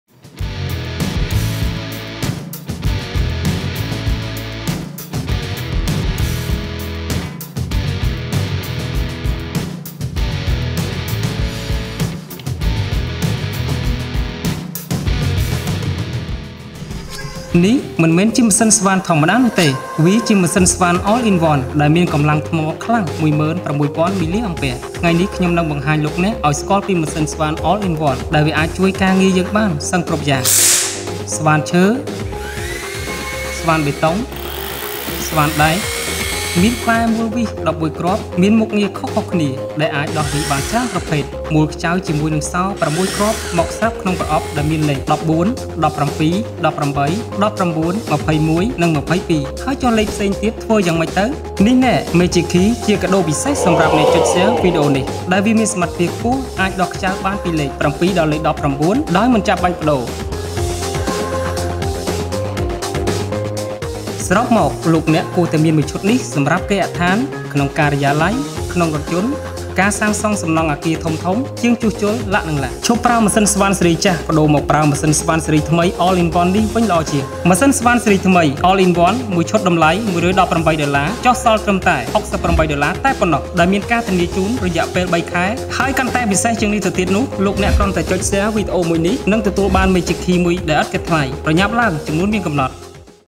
ម៉ូទ័រស្វាន All in One​ ​តំលៃ$1​18 ចុះសល់$68 (Shopi Mall)
🔥🎉ម៉ូទ័រស្វាន All in One​ ​តំលៃ $1​18 ចុះសល់ $68 និង​ថែមជូនMagic Key មួយដែលអាចដោះខ្ចៅបានពីលេខ 7 ដល់ 19ផងដែល រកឯណាមានកំលាំងថ្ម 16000mA
👉មួយឈុតមាន៖
-ថ្មរ១គ្រាប់ឆ្នាំងសាក1
-ក្បាល់ទុលរវិសតូច1ឈុតមាន10គ្រាប់ និងវែង 1គ្រាប់
-ផ្លែស្វាន1 និង ផ្លែស្វានប៊ីតុង1
- សោដោះកង់ឡាន 6គ្រាប់ លេខ 14/17/18/19/21/22
- ខ្លាញ់គោ1ដប់ និងខ្សែក្រវ៉ាត់ស្ពាយម៉ូទ័រមួយ
🎖📦 ធានារយះពេល 3ខែ
📱☎️ លេខទូរស័ព្ទ ៖ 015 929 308​ ​/ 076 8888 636 / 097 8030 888
🚛📦​ សេវាកម្មដឹកជញ្ជូនដោយឥតគិតថ្លៃ!!